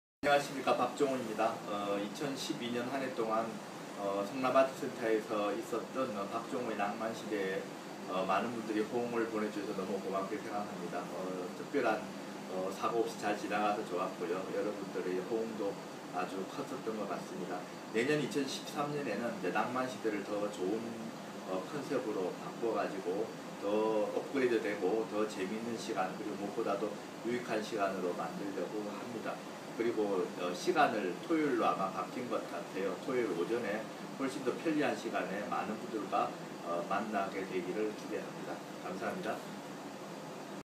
2012년 박종호의 낭만시대 감사 인사
2012년도 공연을 마무리하는 소감 그리고 2013년도 공연 소식을 살짝 말씀해주시는 박종호 선생님의 인터뷰입니다..